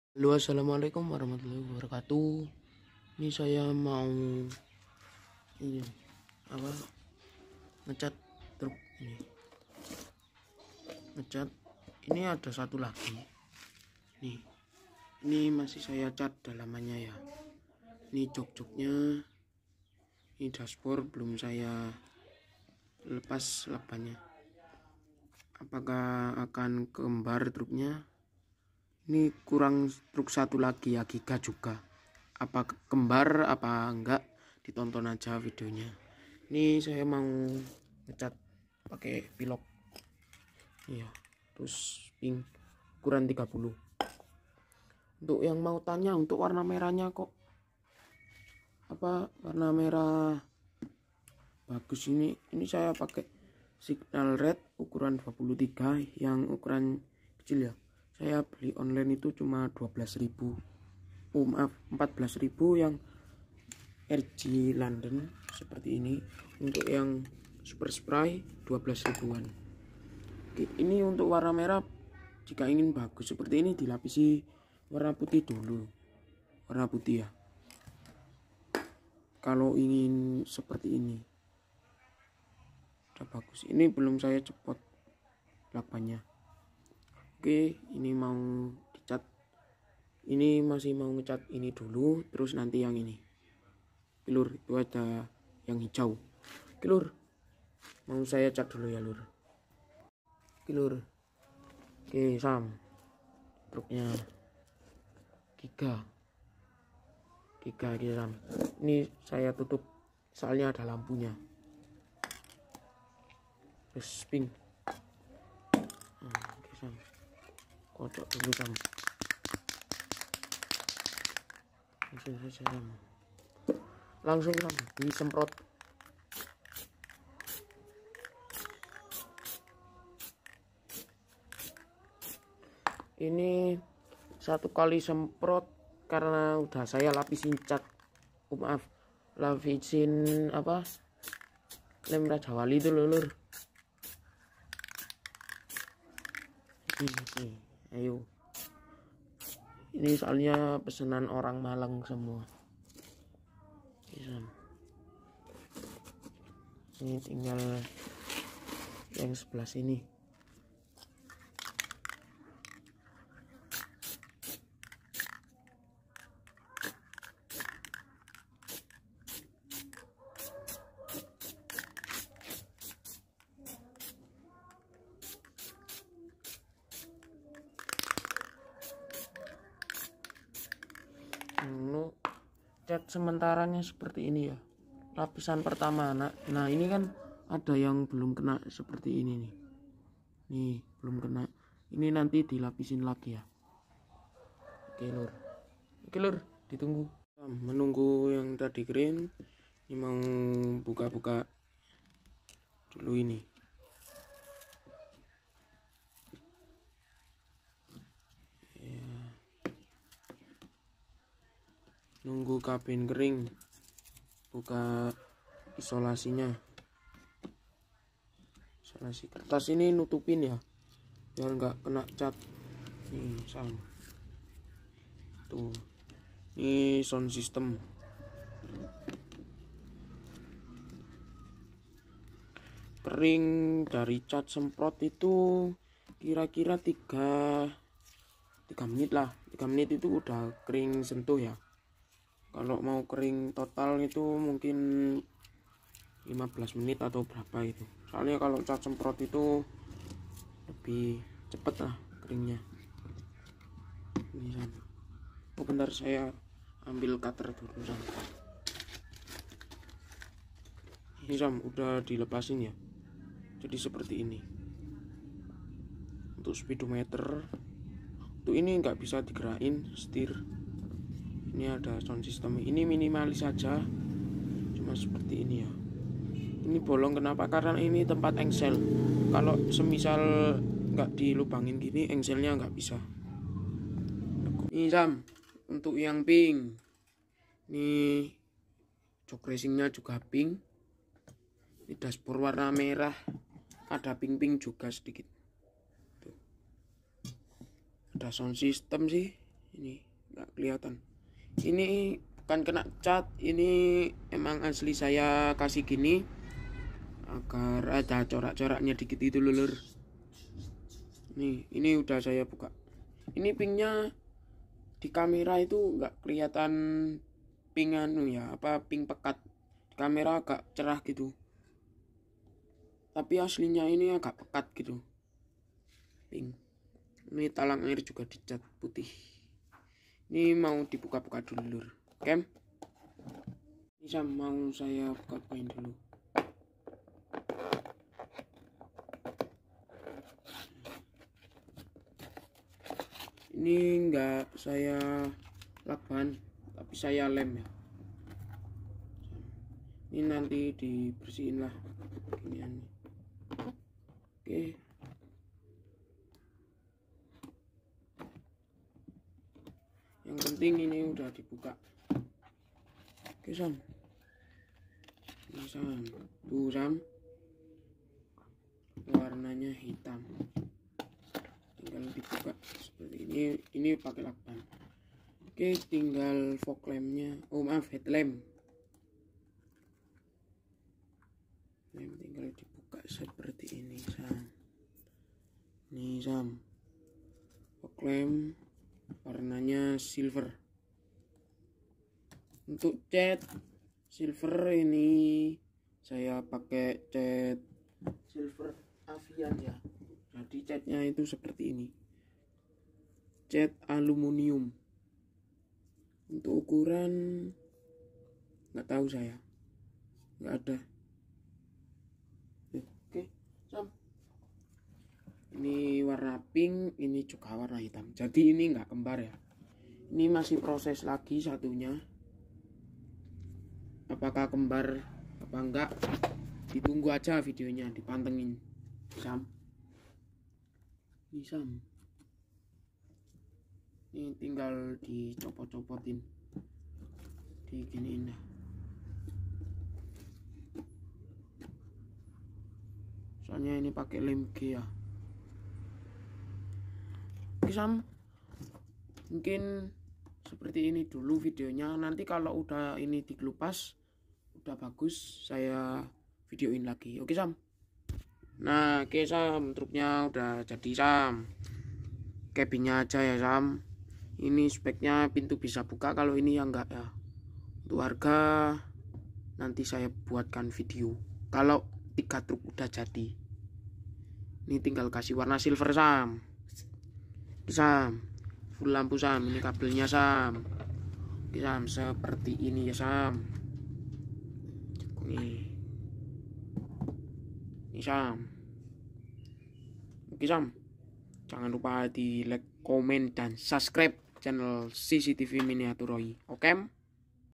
proses pengecatan kabin truk kembar??
simak vidio nya lird apakah truk nya jadi kembar apa nggk.